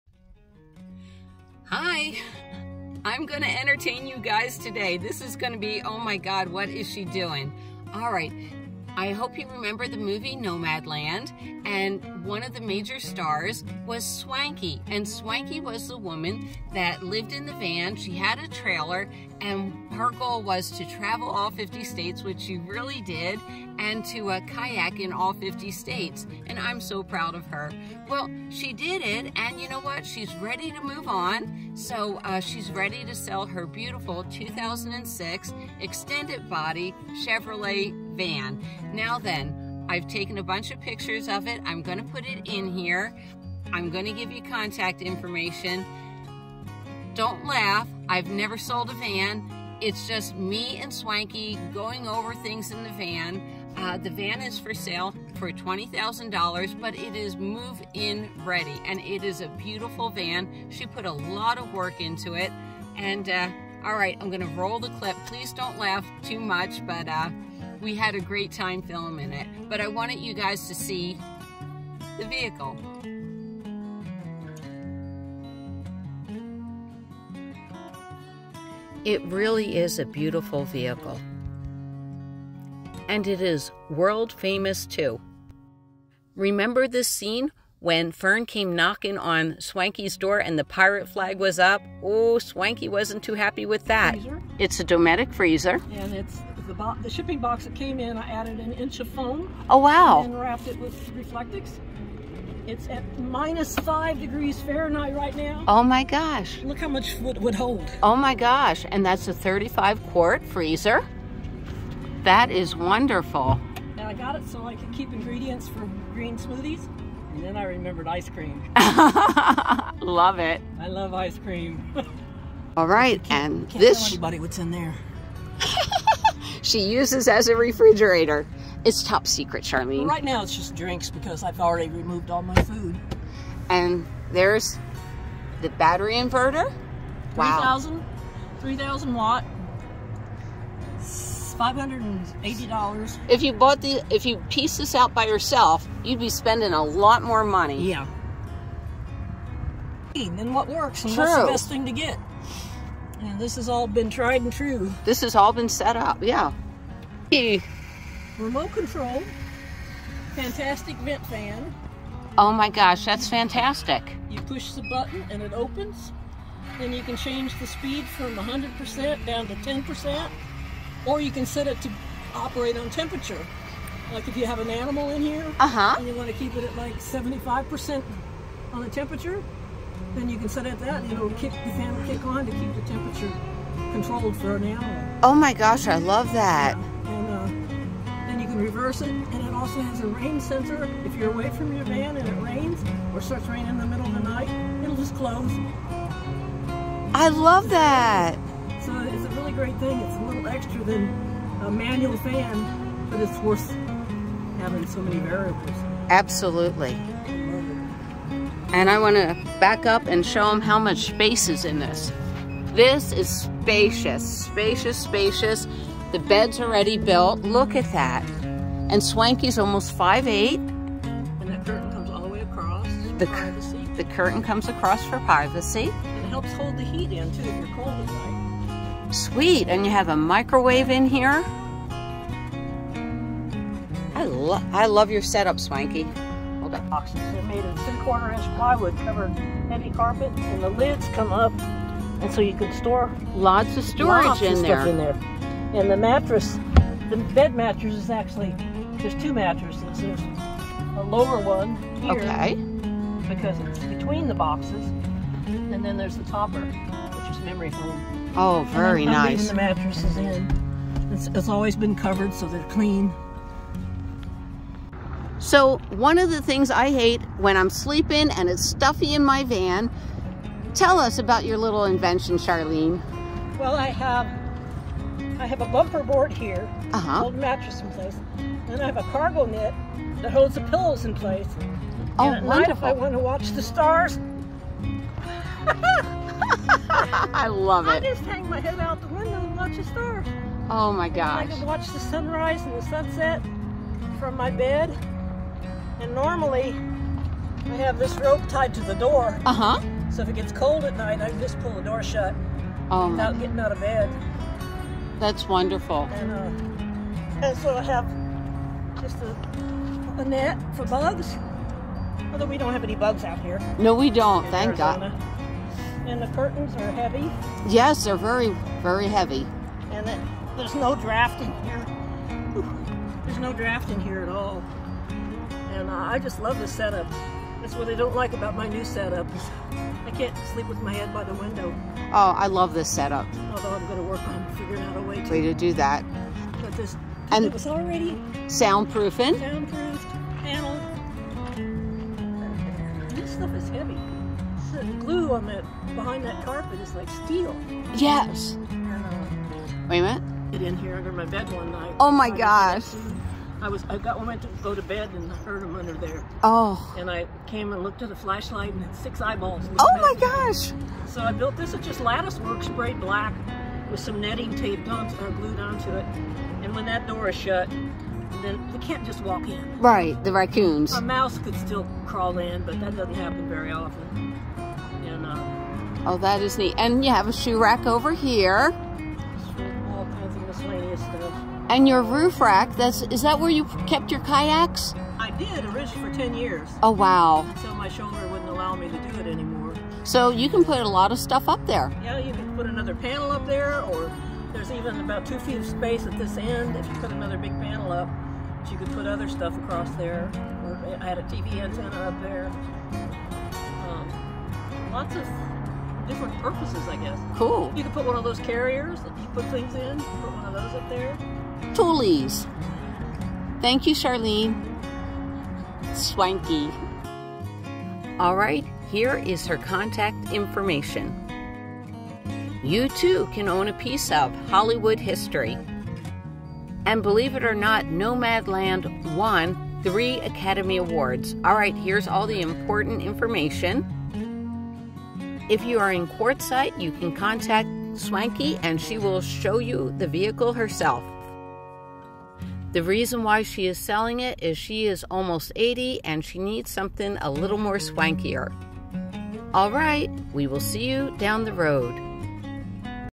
SOLD! Buy Movie History! Nomadland WON 3 Academy Awards Swankie is Selling her Move-In Ready Van!
1752 E. Lugonia Ave. 117-9